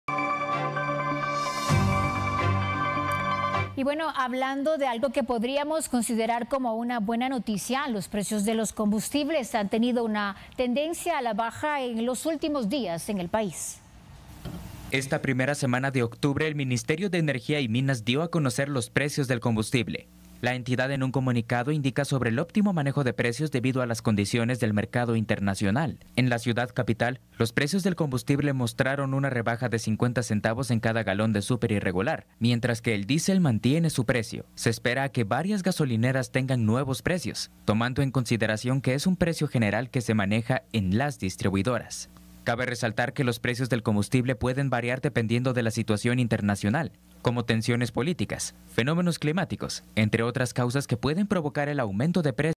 Precio de combustible baja notoriamente en el país
Cabe resaltar que el costo depende también de las distribuidoras.